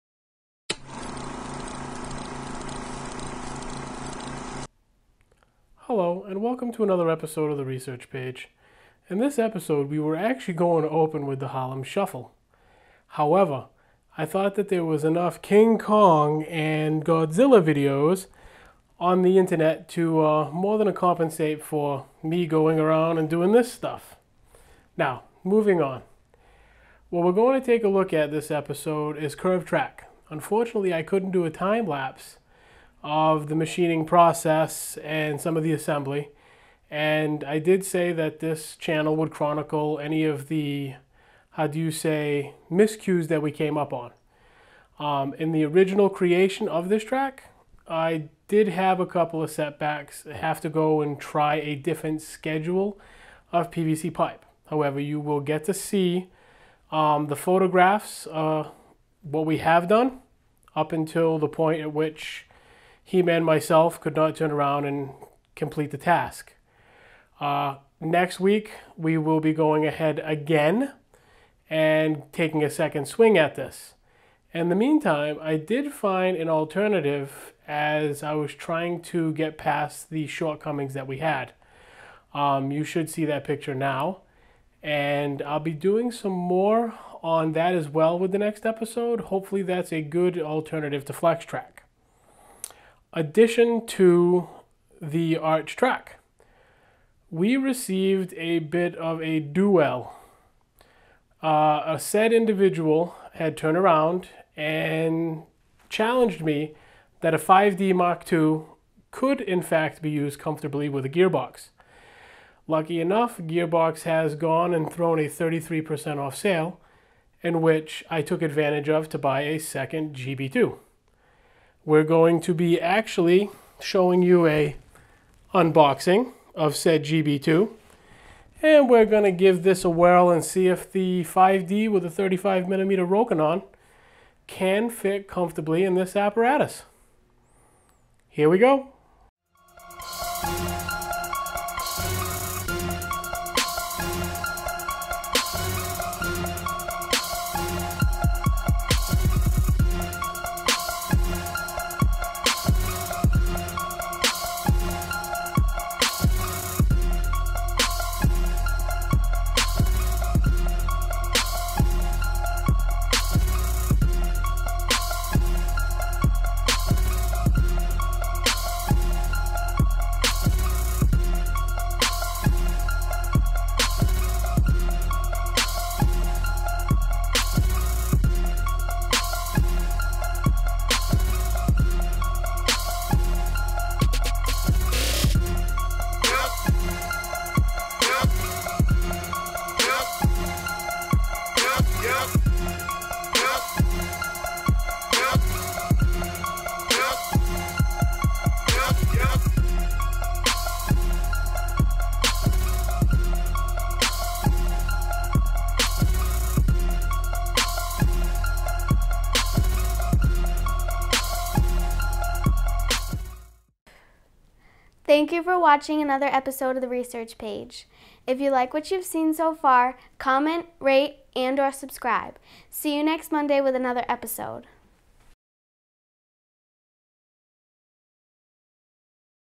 TRP Curved Track and Great GearBox Challenge
In this episode of TRP we take a swing at Curved Dolly tracks DIY style with some sample footage as well ass address a few comments in regards to the P & C gearbox and it's ability to accommodate a Canon 5D MKII without the need for the useless spacers they provide.